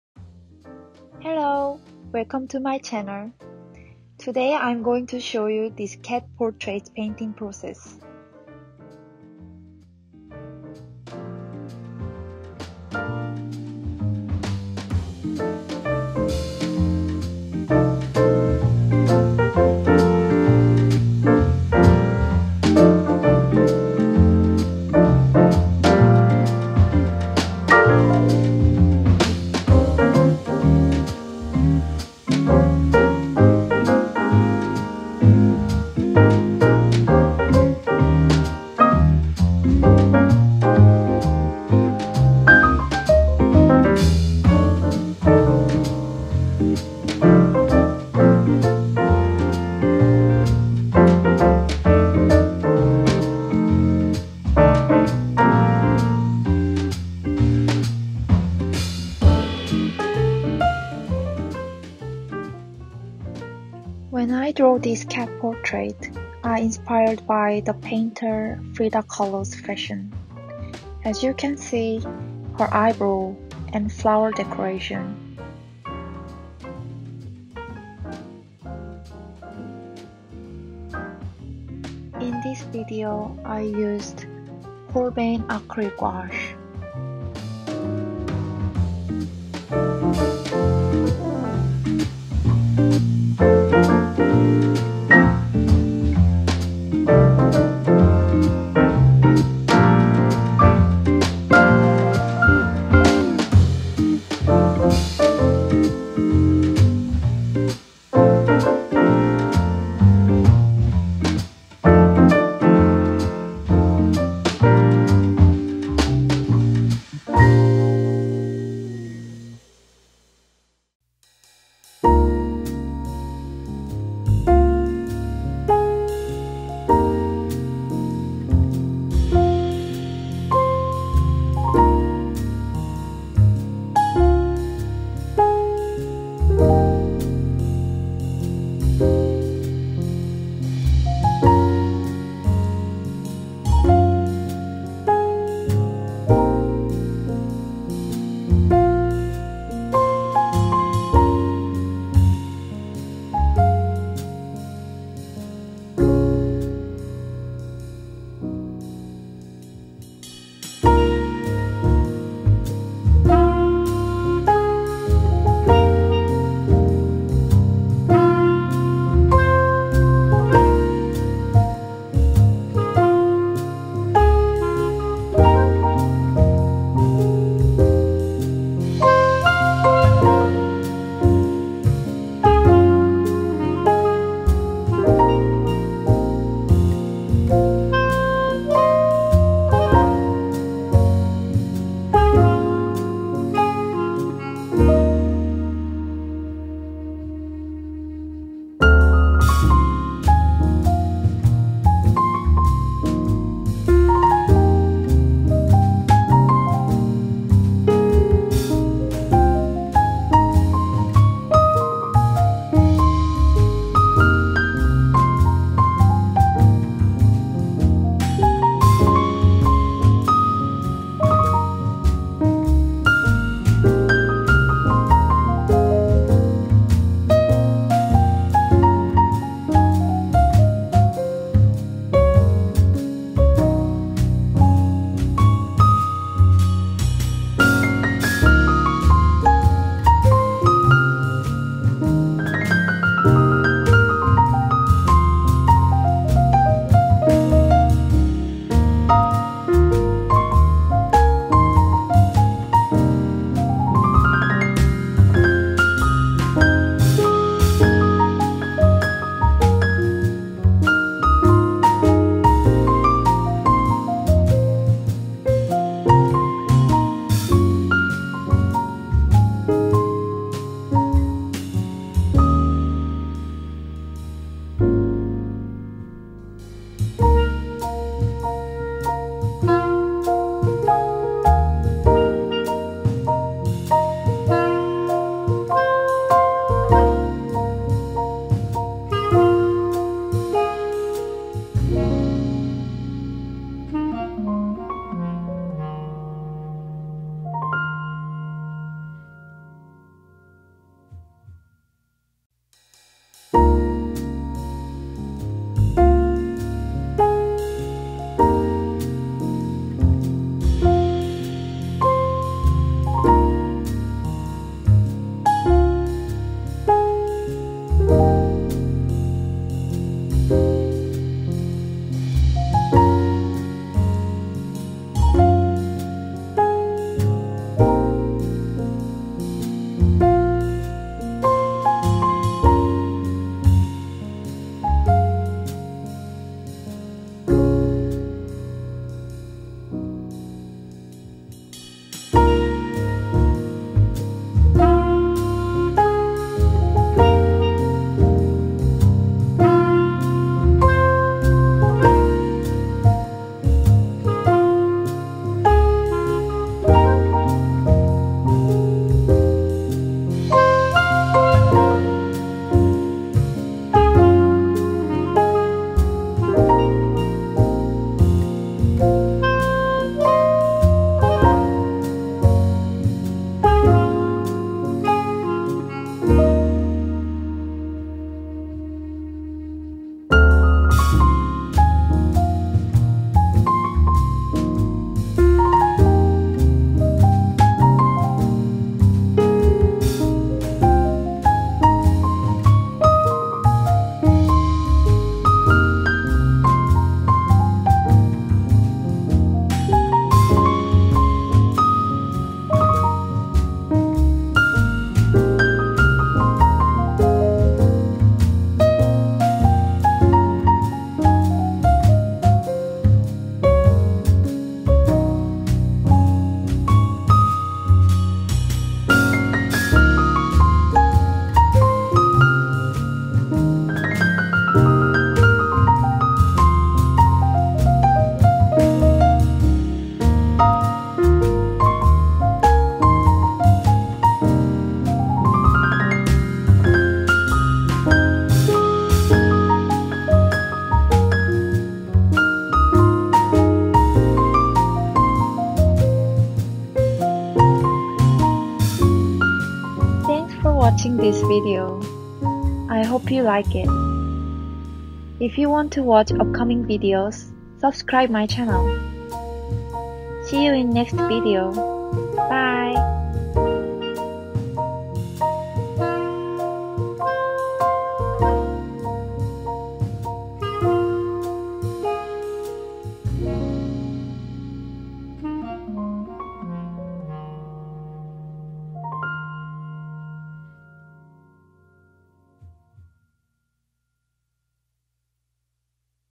🎨Draw with me! #2 Cat portrait, Frida Kahlo/ with Acrylic gouache paint on watercolor canvas
This video is about the painting process of the cute cat portrait.

*FOLLOW ME*

Camera
• iPhone 11

Art supplies
• Holbein acryla gouache
• Phoenix artist watercolor canvas professional 10cm x 10cm
• Hwahong brush 345 no.4
• Kum memory point brush no.8